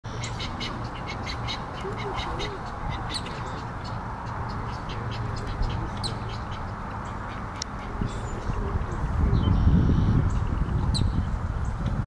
Baby Egrets with parents at Venice, FL Rookery
Baby Egrets in a rookery in Venice Florida